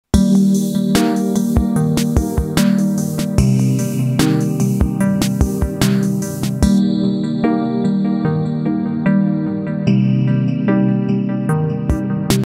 Collision Detection, Animation, and OOP in javaScript
A video of the final version of a javaScript demo using the p5.js library. Learn how

When the objects collide, a connecting line is rendered between them. If the colliding objects are each other's first collision, then a blue connecting line will be drawn instead of white. And there are baby orbiting objects too, just because. Enjoy x